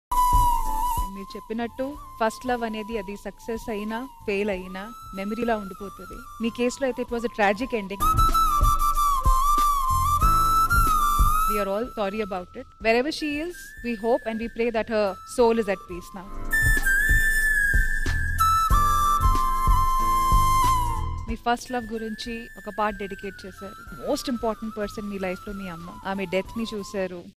Dhee Pandu Speech about his mom and Love
Dhee Pandu Speech about his mom and first love missed in his life.